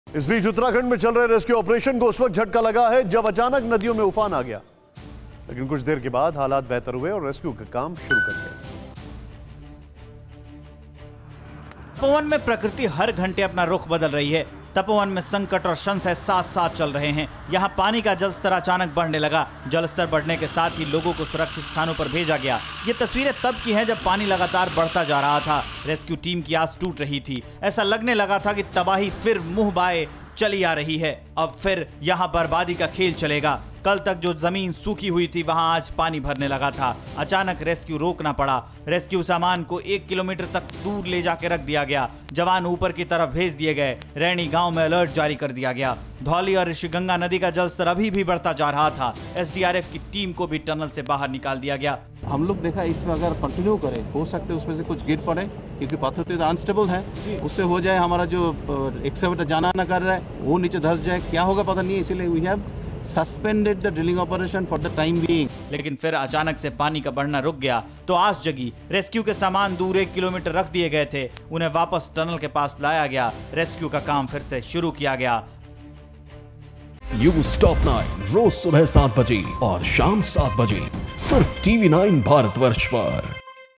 98 घंटे बाद रेस्क्यू विराम.. फिर से शुरू हुआ Chamoli में बचाव अभियान
उत्तराखंड में चल रहे रेस्क्यू ऑपरेशन को उस वक्त झटका लगा जब अचानक नदियों में उफान आ गया. लेकिन कुछ देर के बाद हालात बेहतर हुए और रेस्क्यू का काम शुरु कर दिया गया.